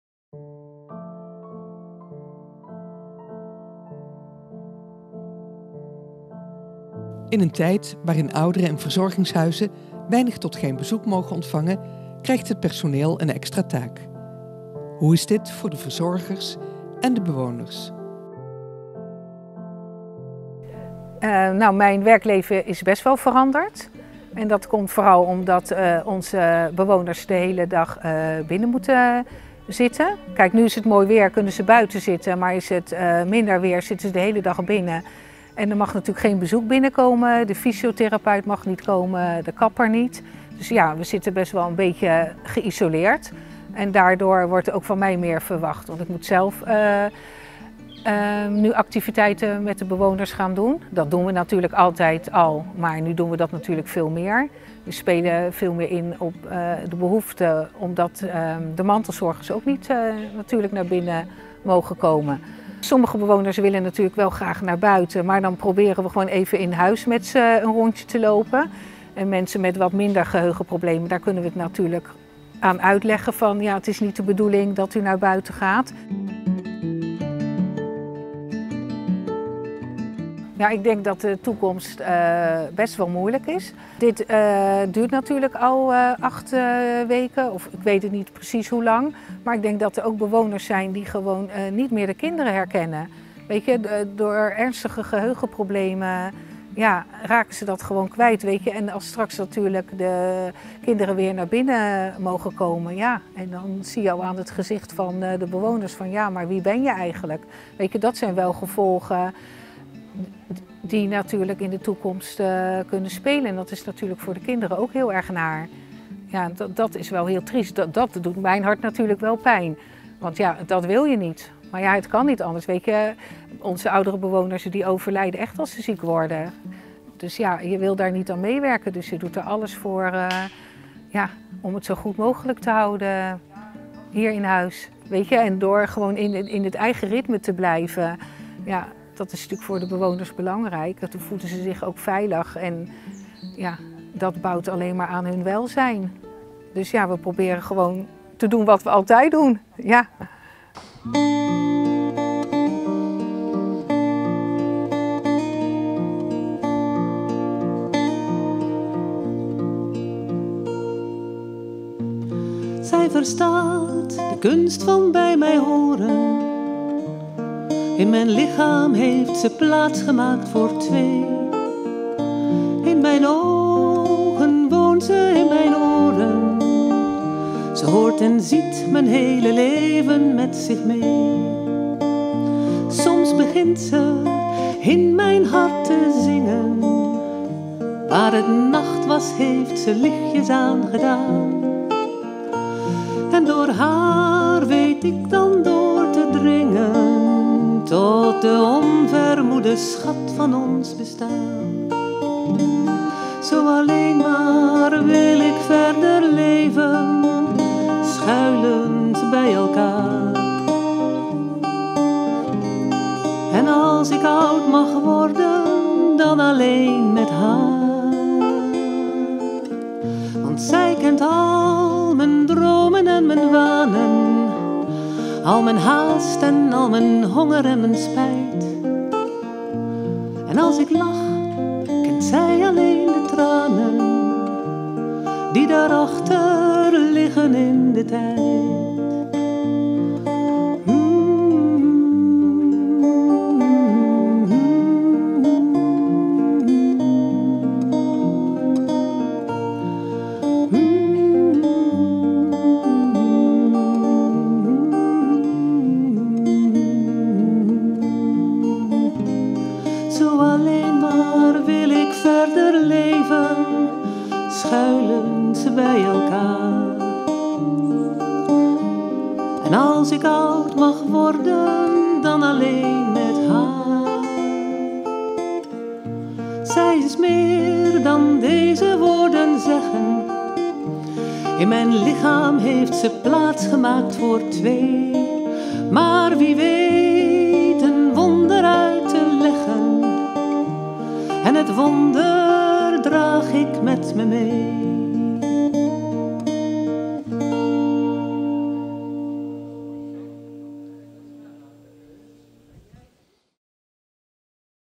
Documentaire 'Corona in Delft': De Herbergier
Stichting Delft-in-Ovatie maakte een documentaire over Corona in Delft en de harde werkers in onze stad 💙  De komende weken delen we clips uit deze documentaire op ons YouTube-kanaal. Met deze keer zorgpersoneel en bewoners van De Herbergier aan het woord.